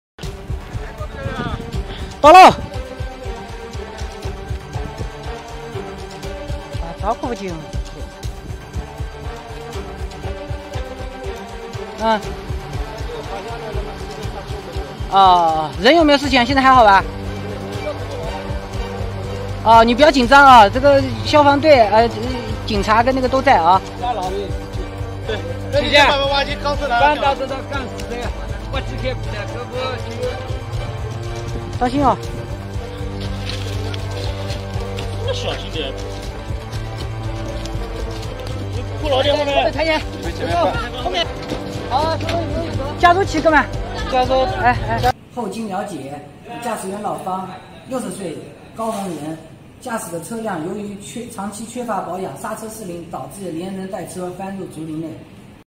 【1818黄金眼】货车翻下山坡，30吨重车身压住司机左腿
【货车翻下山坡 30吨重车身压住司机左腿】18日上午，临安一名货车驾驶员在倒车时，因为货车长期缺乏保养导致刹车失灵，连人带车翻下山坡。身材瘦弱的驾驶员老方上半身卡在车内，左腿被压无法动弹，事发地点又是一片竹林，救援人员克服困难，花了一个多小时将他抬下山送医救治。幸运的是，老方只有些皮外伤和腰部肌肉轻度扭伤，其他并无大碍。

新闻标签：